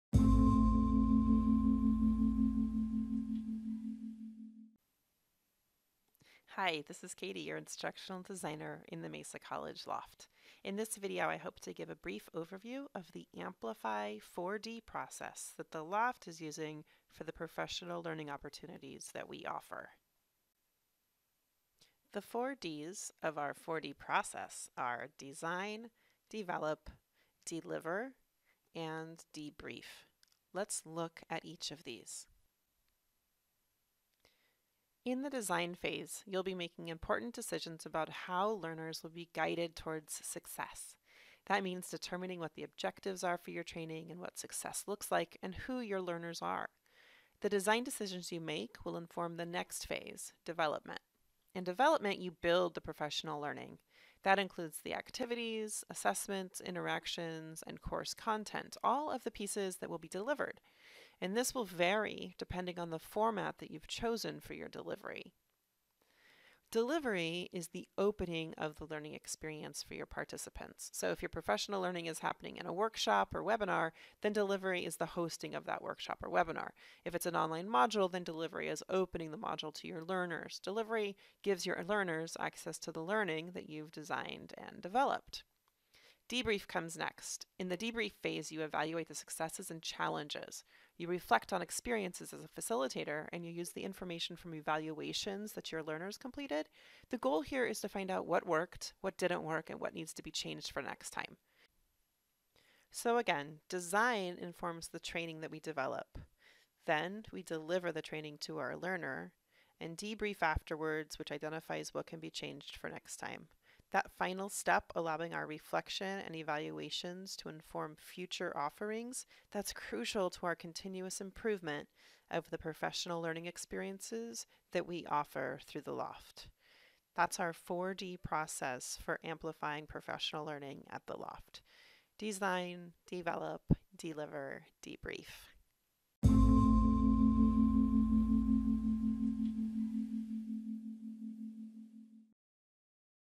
Mesa LOFT 4D Amplify
This video gives a brief overview of the Mesa LOFT Amplify 4D process.